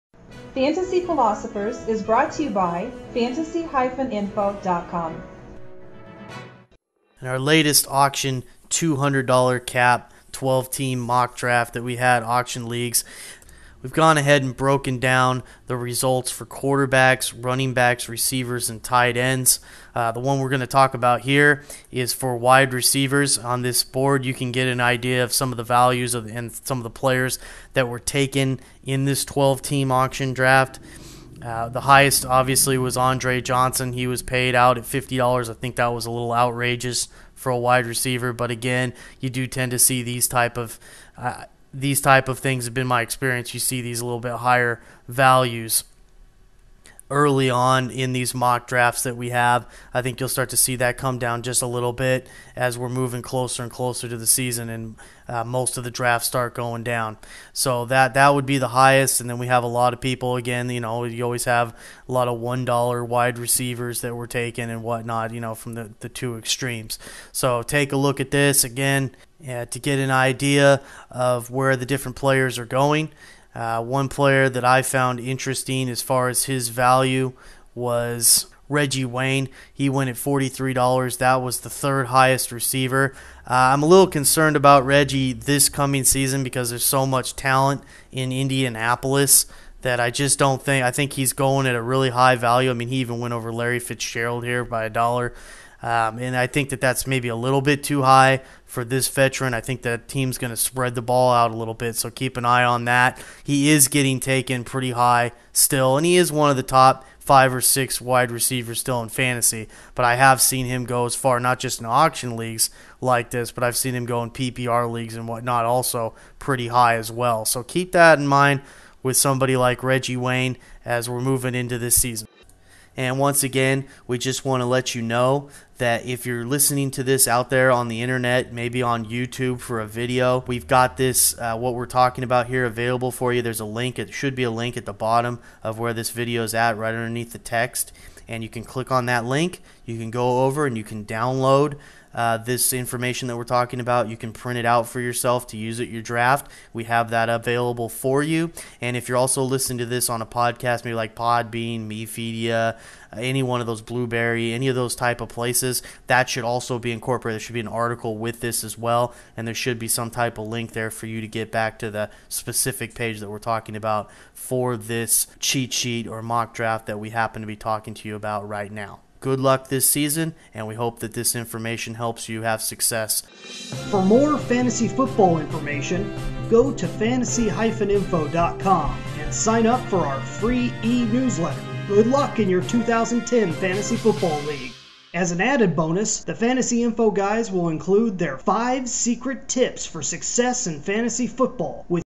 Results for Wide Receivers (WR) -- 2010 12 Team Auction Fantasy Football Mock Draft
Welcome to our wide receiver breakdown results page for our Auction fantasy football league snake mock draft. As in all fantasy drafts, many wide receivers are selected and this sheet can be useful to get an idea of where receivers are going throughout the draft.